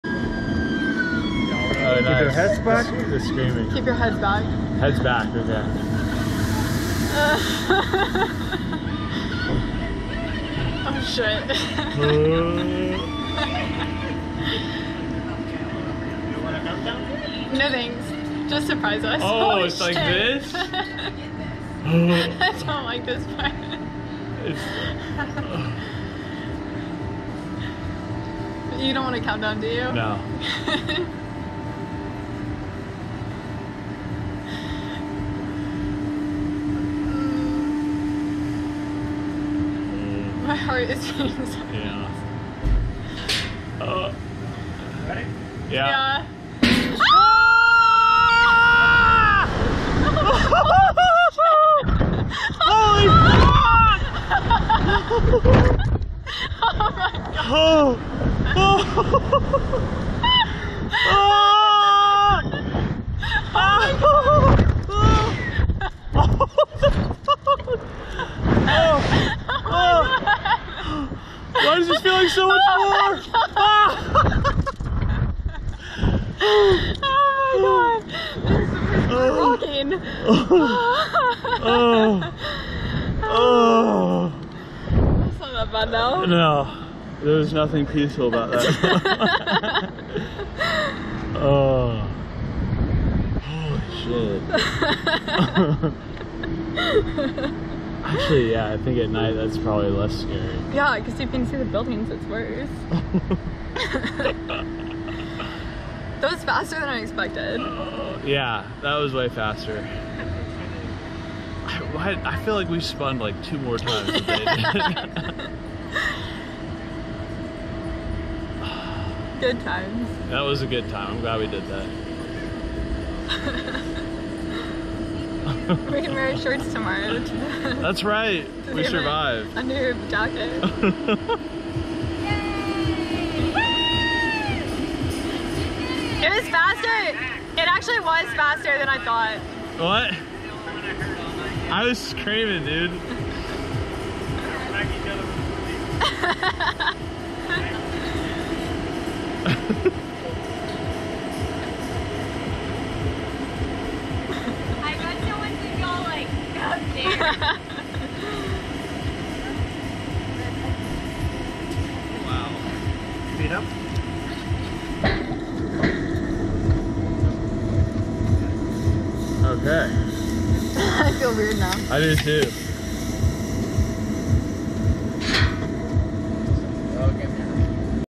Tinsley & Mackenzie
400' tall attraction that launches riders at 100 m.p.h.